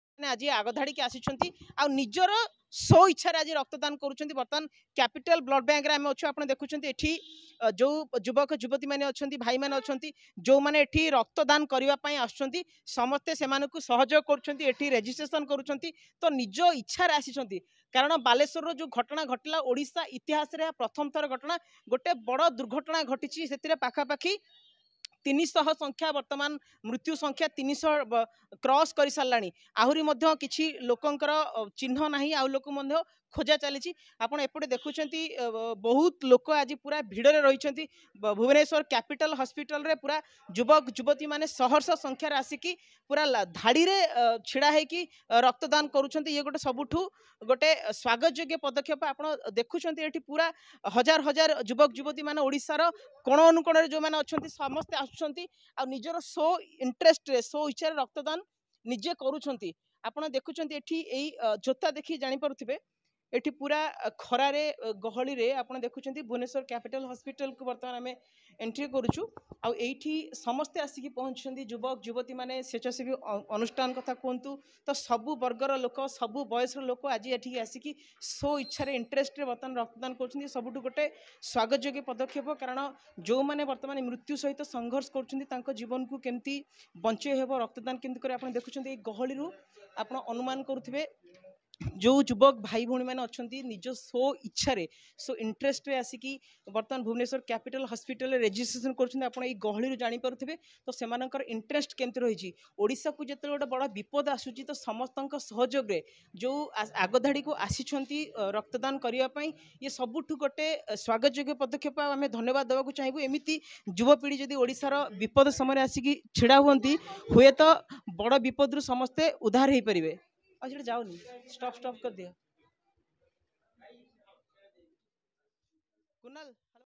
ବାଲେଶ୍ୱର ଦୃଘଟଣା ରେ ଆହତ ରୋଗୀଙ୍କୁ ରକ୍ତ ଦେବା ପାଇଁ ରକ୍ତଦାତାଙ୍କ ଲମ୍ବା ଲାଇନ କ୍ୟାପିଟାଲ ବ୍ଲଡ଼ ବ୍ୟାଙ୍କରେ
ବାଲେଶ୍ୱର ଦୃଘଟଣା ରେ ଆହତ ରୋଗୀଙ୍କୁ ରକ୍ତ ଦେବା ପାଇଁ ରକ୍ତଦାତା ଙ୍କ ଲମ୍ବା ଲାଇନ କ୍ୟାପିଟାଲ ବ୍ଲଡ଼ ବ୍ୟାଙ୍କ ରେ